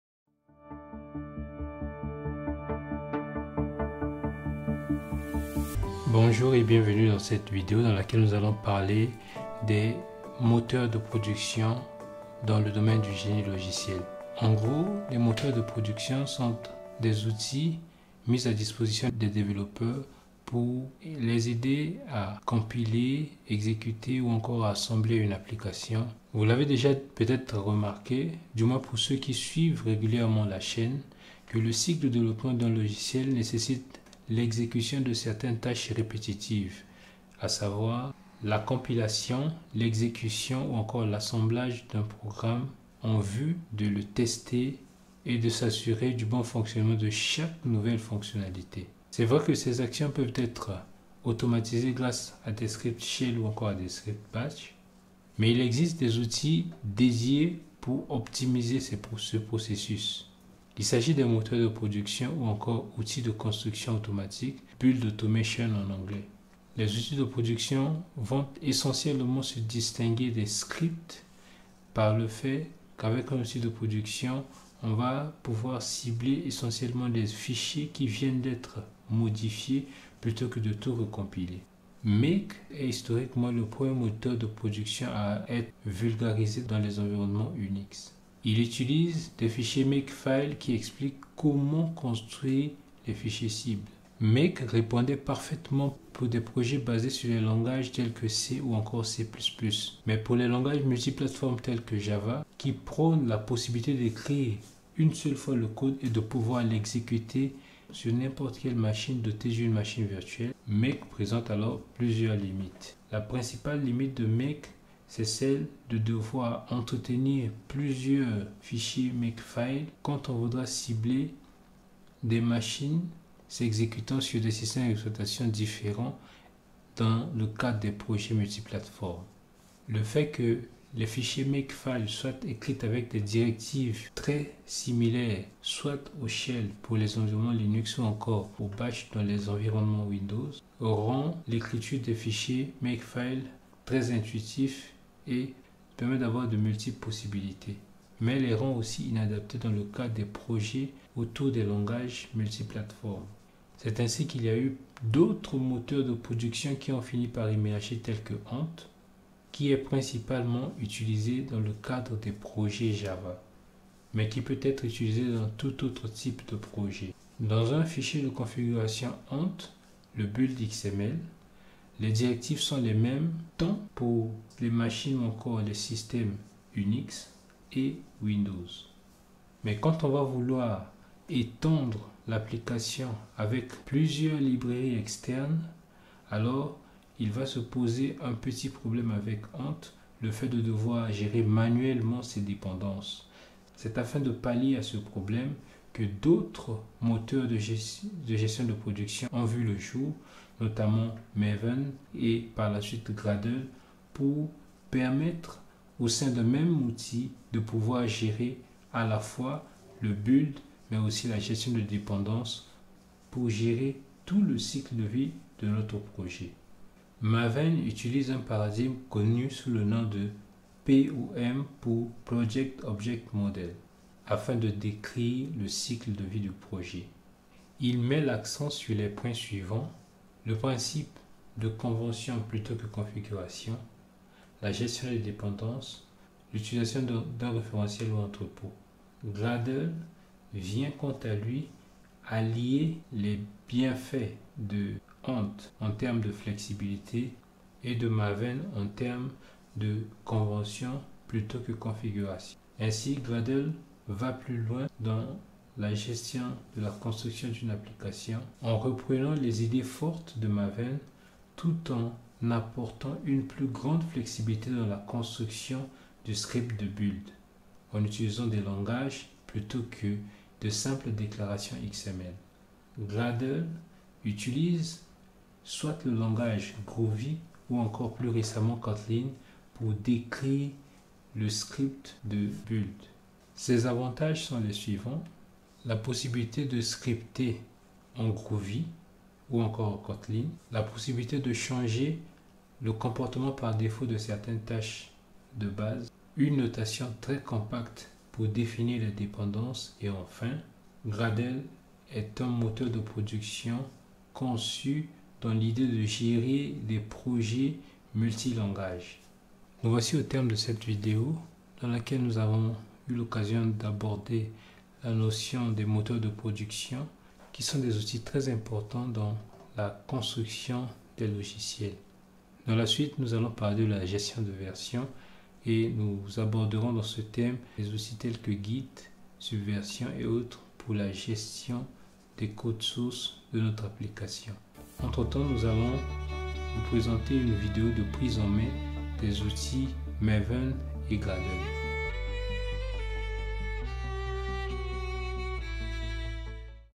Les moteurs de production - 5 min pour découvrir le génie logiciel
Cette vidéo parle de moteurs de production ou outil de construction automatiquement de logiciel, build automation en anglais.
Un moteur de production se distingue d'un simple script dans la mesure où il a pour objet :
 de favoriser une production optimale (réduite aux seules actions nécessaires)

 de permettre une flexibilité de production (production partielle sans avoir besoin de modifier le descriptif de production) tout en assurant la complétude.

En claire, là où un script shell va, par exemple, recompiler tous les fichiers du projet, un moteur de production ne recompilera que les fichiers modifiés.

Nous présenterons les outils  Make, Ant, Maven et Gradle; puis nous en ferons une petite comparaison.